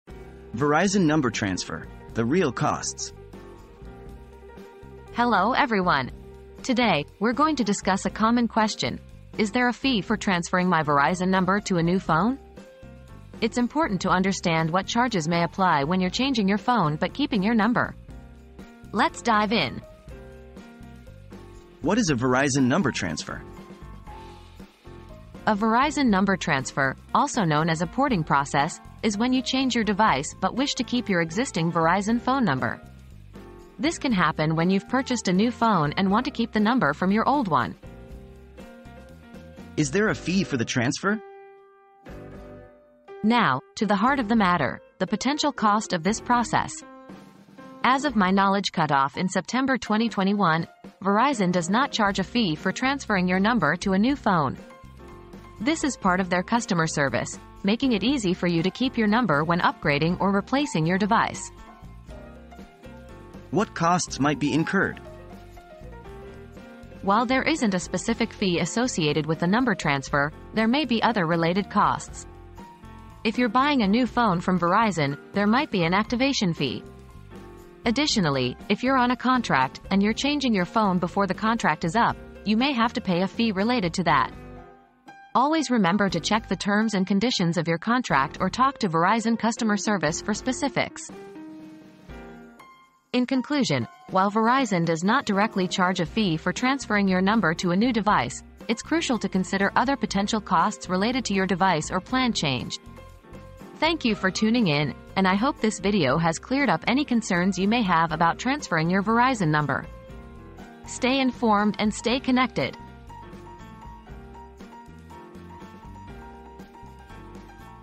Verizon Number Transfer: The Real Costs
Is there a fee for transferring my Verizon number to a new phone? • Curious about the cost of transferring your Verizon number to a new phone? Look no further! Our video breaks down the real costs of this process, including potential fees and charges you may encounter. Stay informed and keep your number with ease - watch now!

00:00 • Verizon Number Transfer: The Real Costs
00:21 • What is a Verizon Number Transfer?
00:39 • Is there a fee for the transfer?
01:03 • What Costs Might Be Incurred?